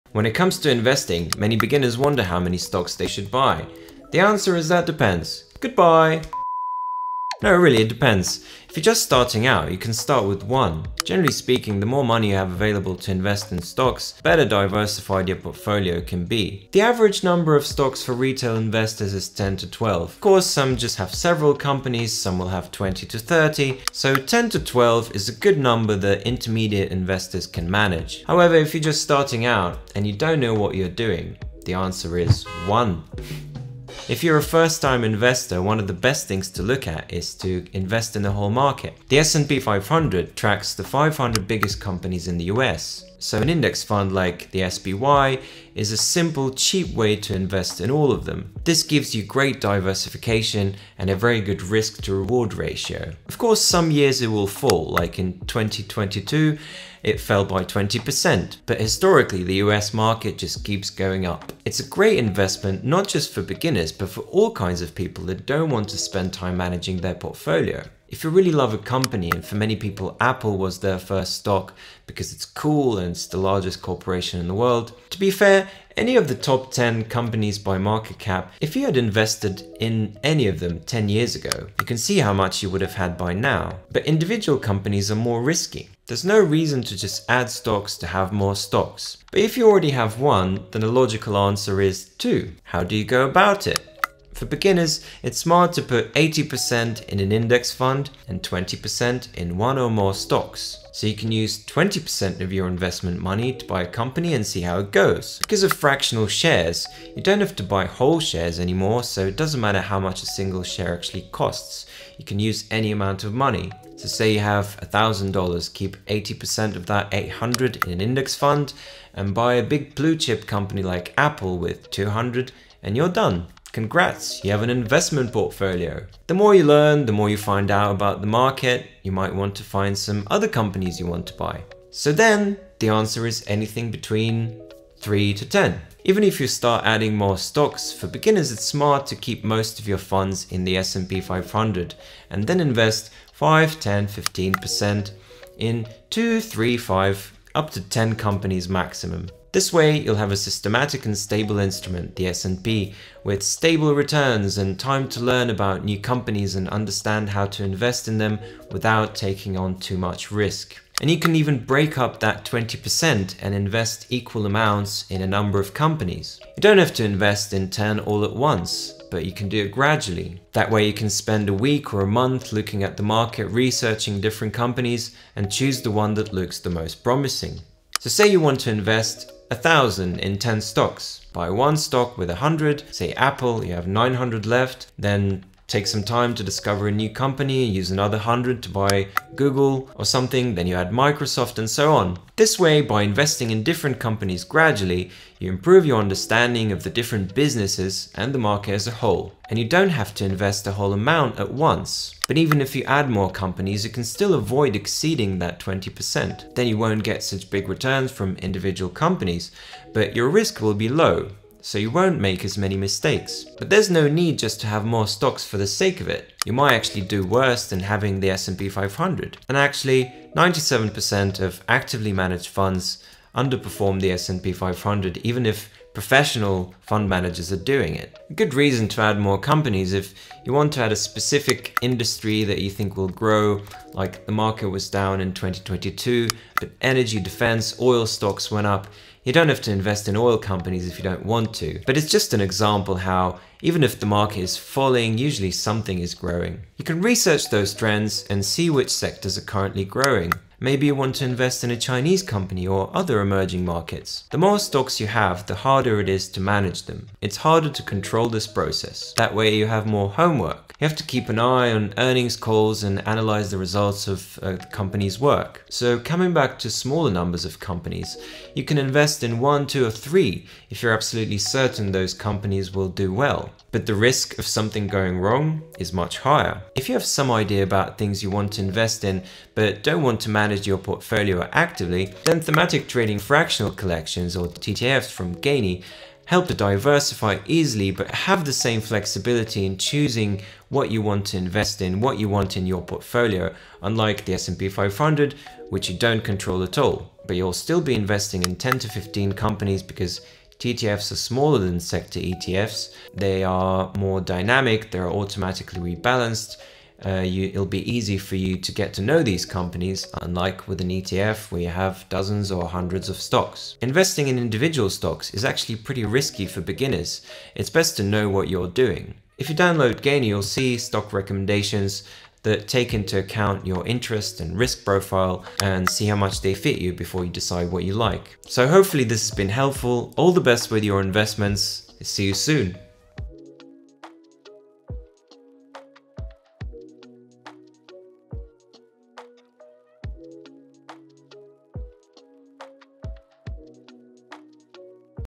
Why You Need Less Than 10 Stocks In Your Portfolio. Investing for Beginners
Are you new to investing in the stock market? Not sure how many stocks to buy or where to start? In this video, we are sharing tips on how many stocks beginners should invest in and provide advice on diversifying your portfolio. Learn more about the S&P 500, individual stocks, Thematic Trading Fractional portfolios (TTFs) from Gainy and understand why individual stock investments can be risky for those who are just starting out.

👉🏼 Download Gainy to invest in themes you like in one